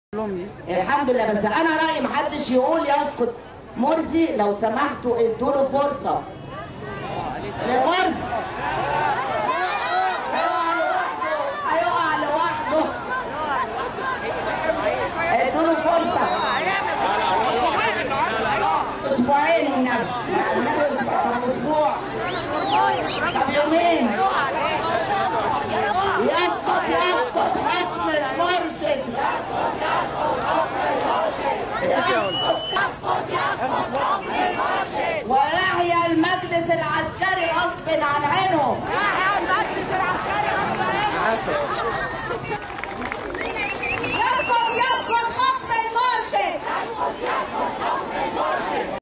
لميس جابر : ماحدش يقول يسقط مرسى هو حيسقط لوحده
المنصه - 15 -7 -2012